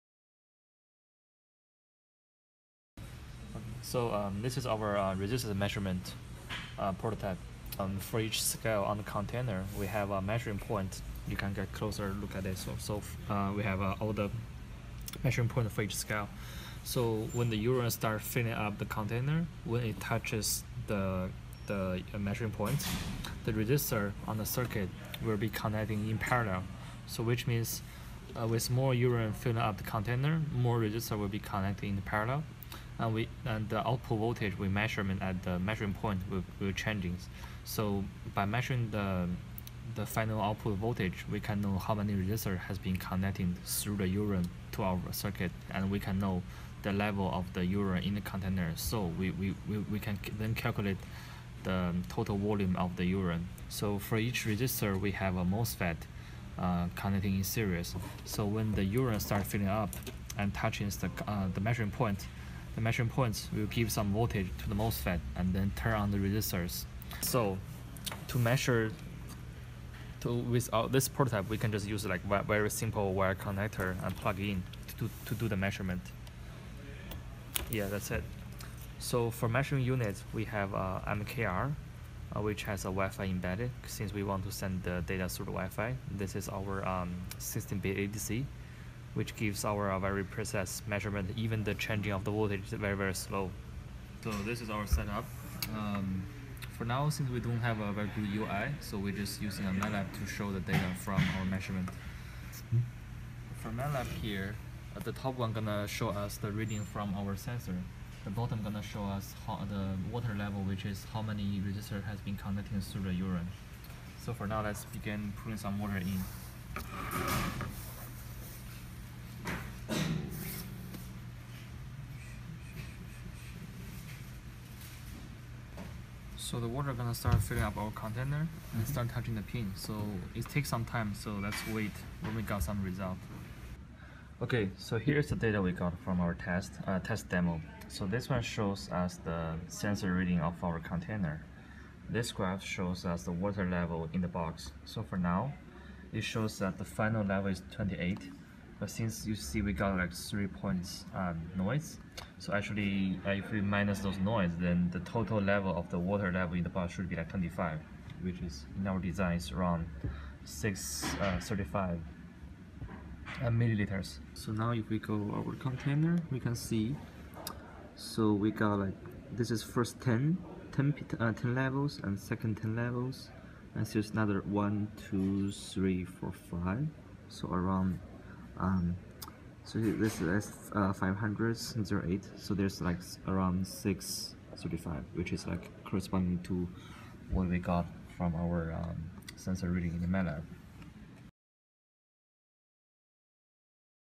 ESE519-Project Video- Digital Urine Meter
This video is a project video for ese519 Real-time Embedded System course at UPENN.
Team Karthik's Cookie Bakery